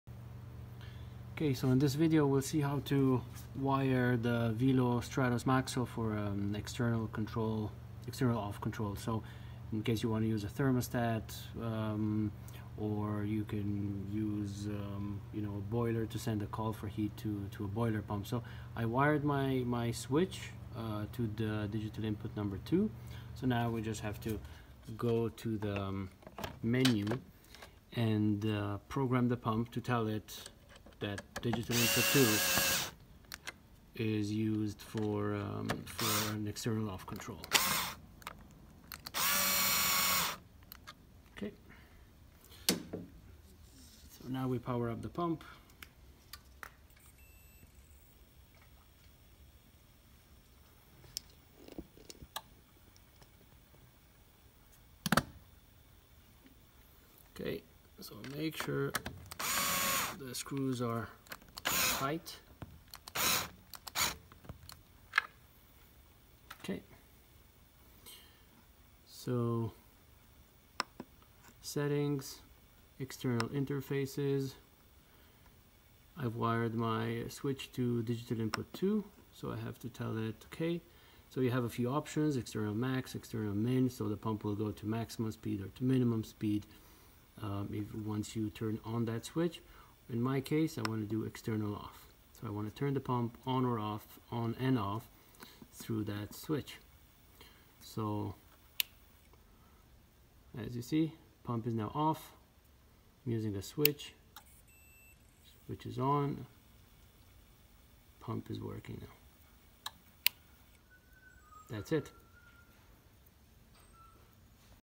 Stratos Maxo external On/Off setup
This video shows how to setup the pump for a external enable signal coming from a controller, thermostat, etc.